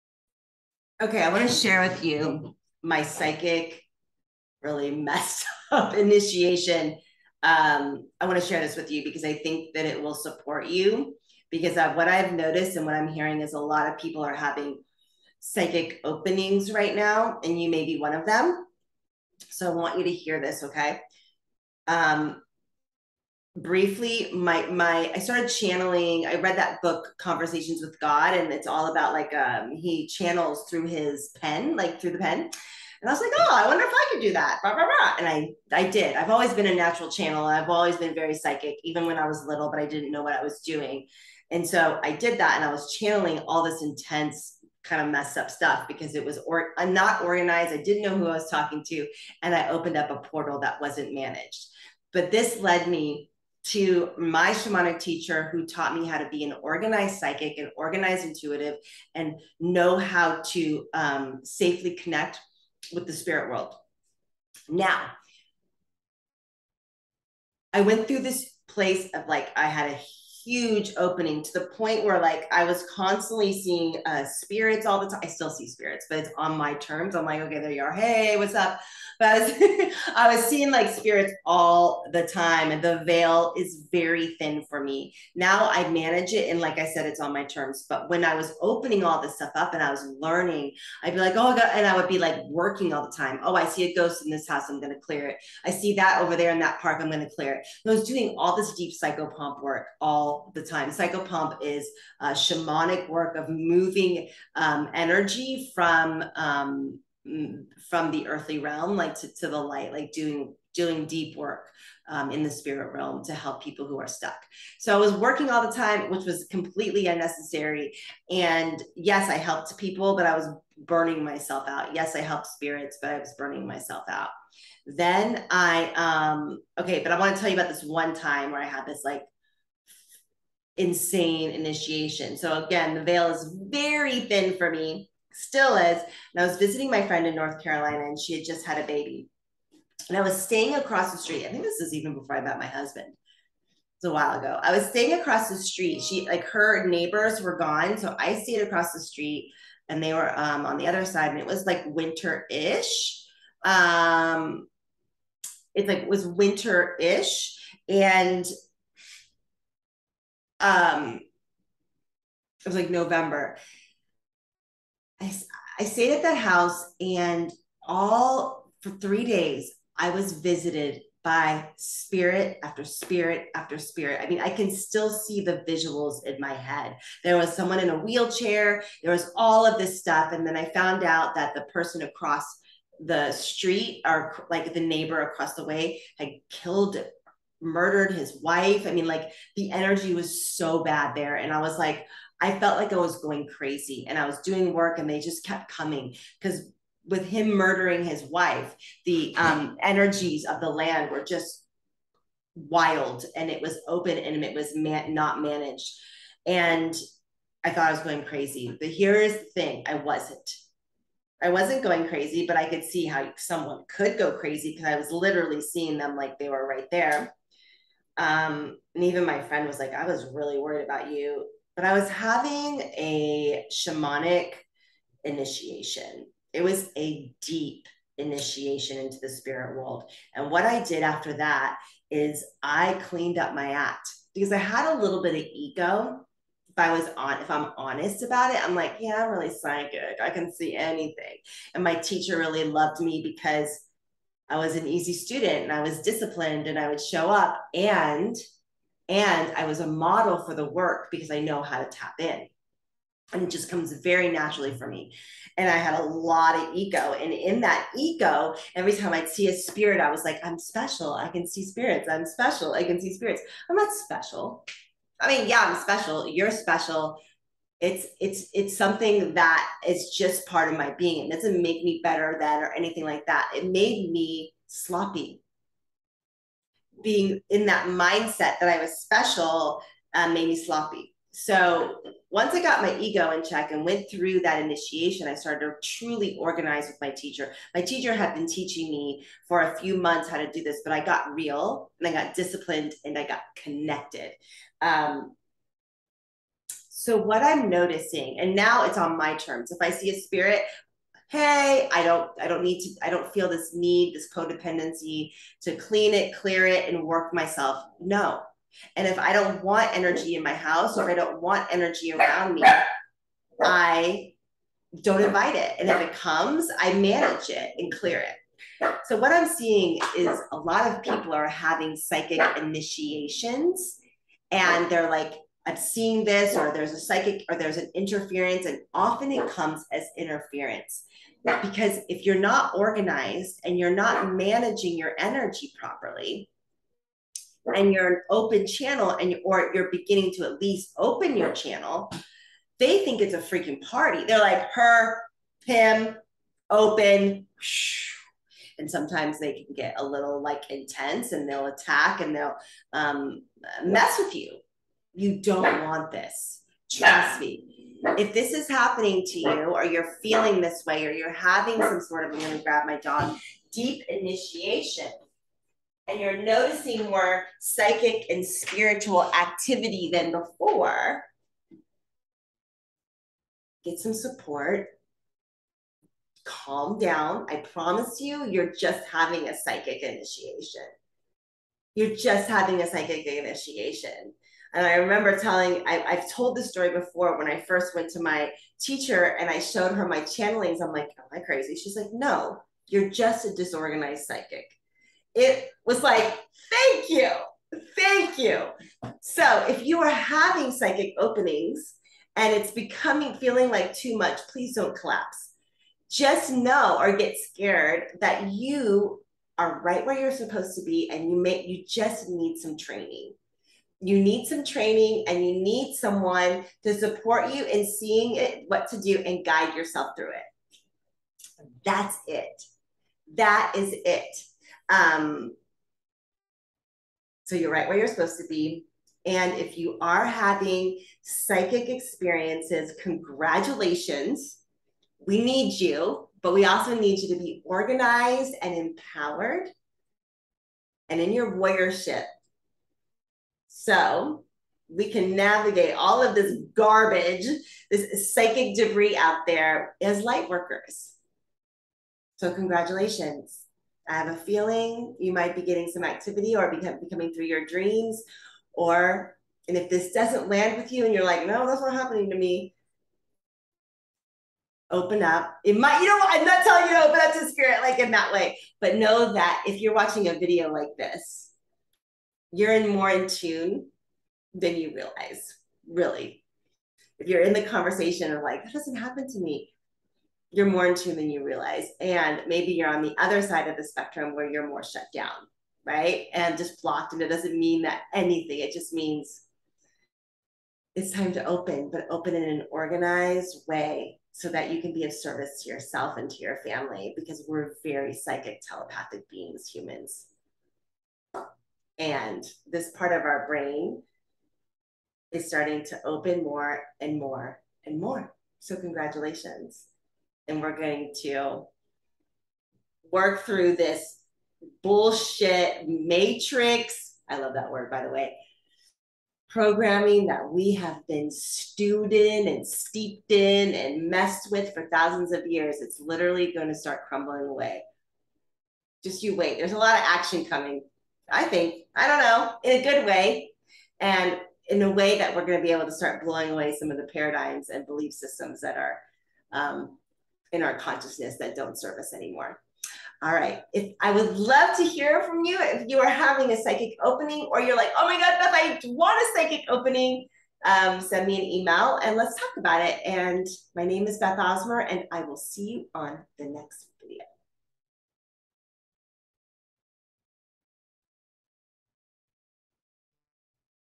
Psychic Initiations Can Be Scary
I have good news for you…

If you are having more psychic and spirit experiences lately, you are exactly where you should be… in fact you are probably in a deep initiation.

If you are wanting connection with spirit and you feel let down or blocked, and feeling like you are not having experiences, congratulations you are in the right place.  And you are probably having more connection than you think.

In the video I talk about my scary AF initiation into deep Shamanic work and I share how to stay grounded and supported during psychic openings.